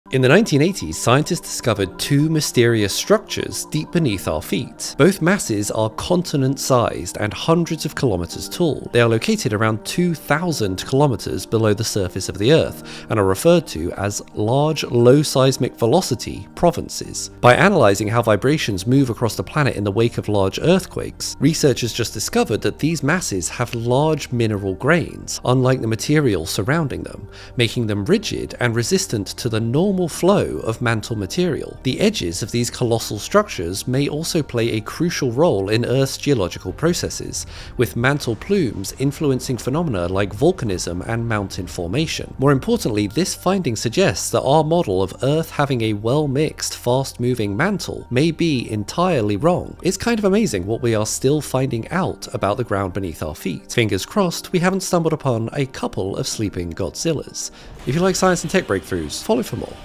Scientists Find Two Huge 'Islands' Hidden Beneath Earth's Surface
Earth has two colossal islands hidden beneath it's crust and scientists just discovered they're more ancient and hotter than previously thought - challenging everything we knew about Earth's mantle.

Camera :
Sony A7III
Lens:
Sigma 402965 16 mm F1.4
Mics: